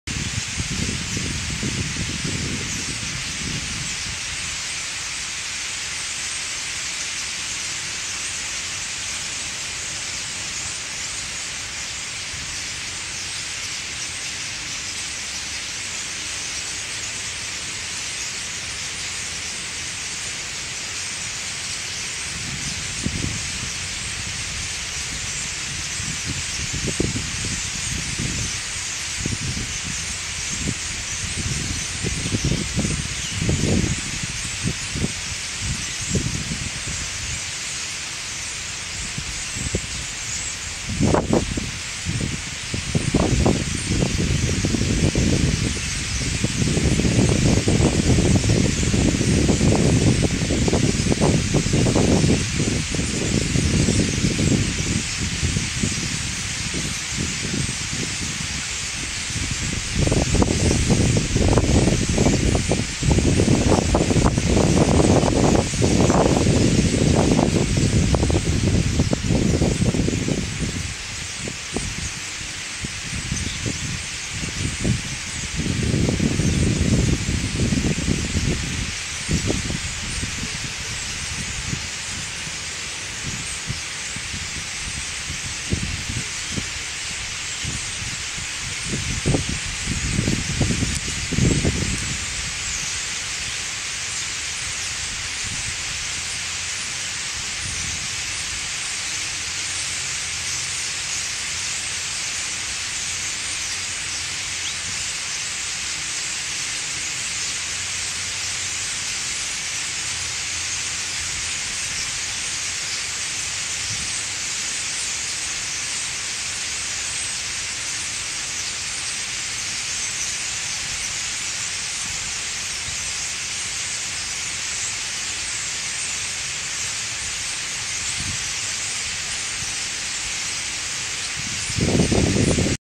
Starling Murmuration - 19th Jan 2018, Leek, Staffs
close view in the trees